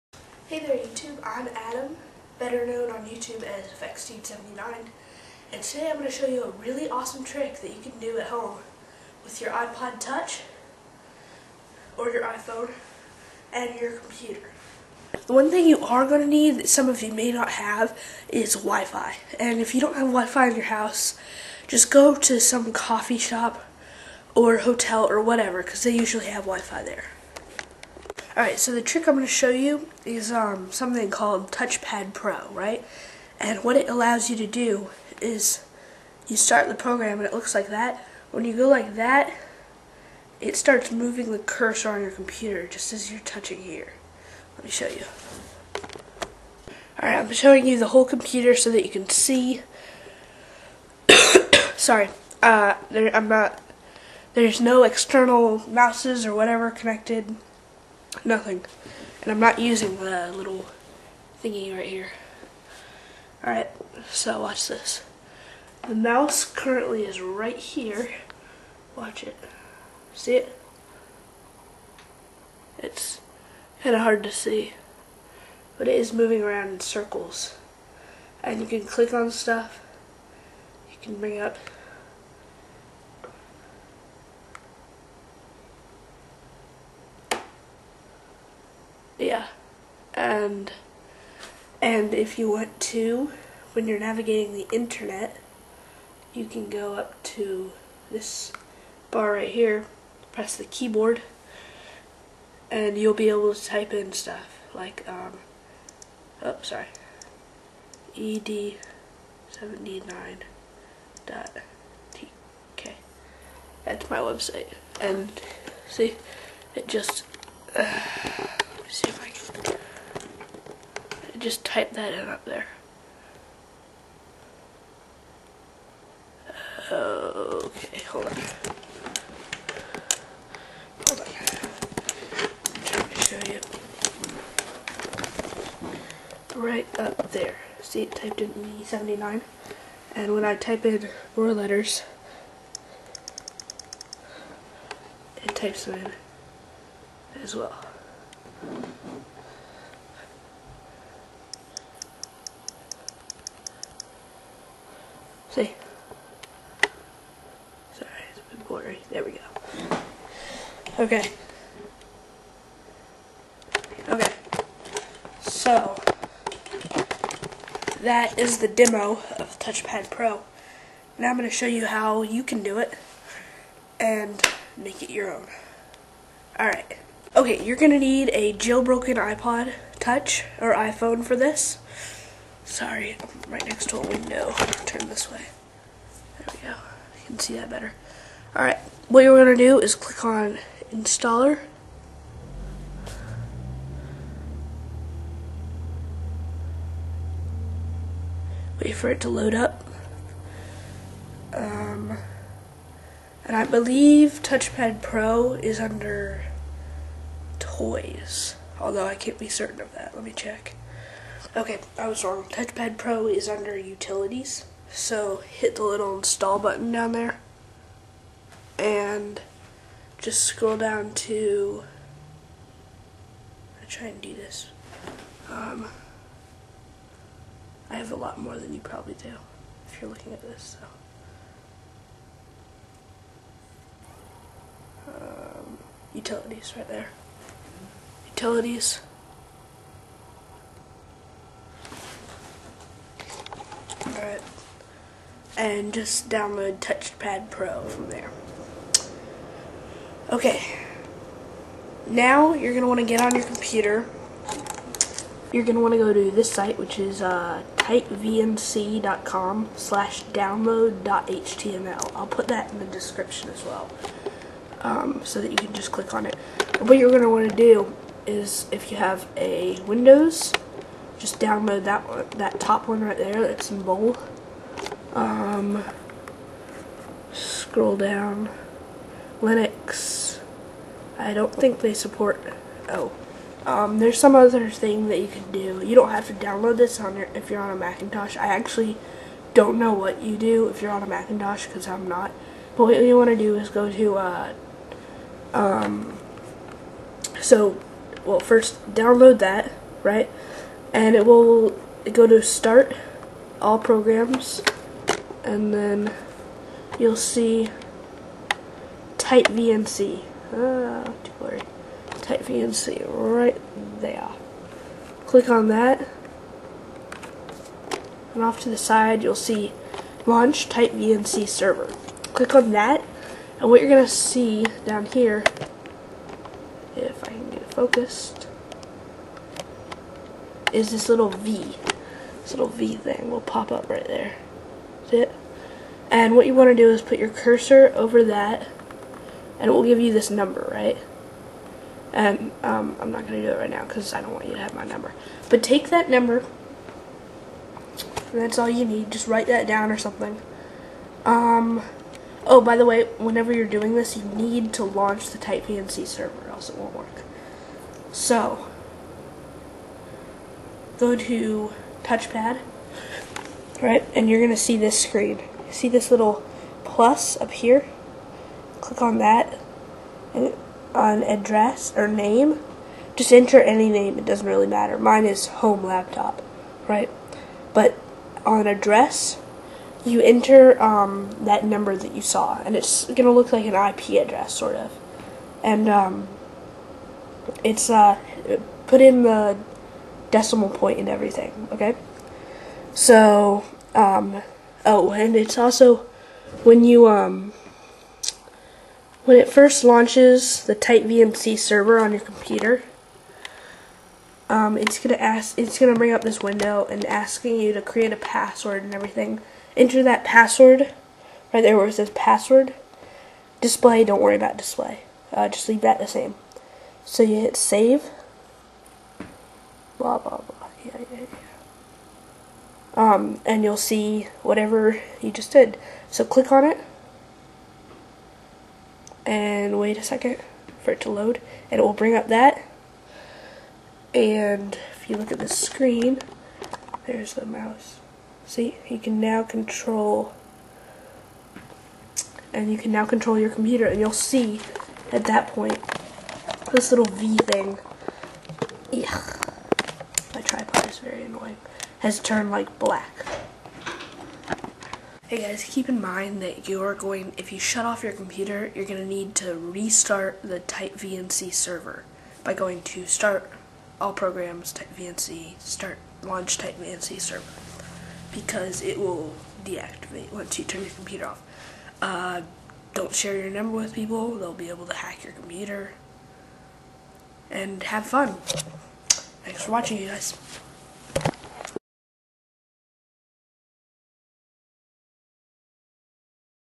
touch pad pro tutorial and demo
download touch pad pro for your ipod touch or iphone and control your computer wirelessly.

how to jail break your ipod touch or iphone (necissary for touchpad pro to work):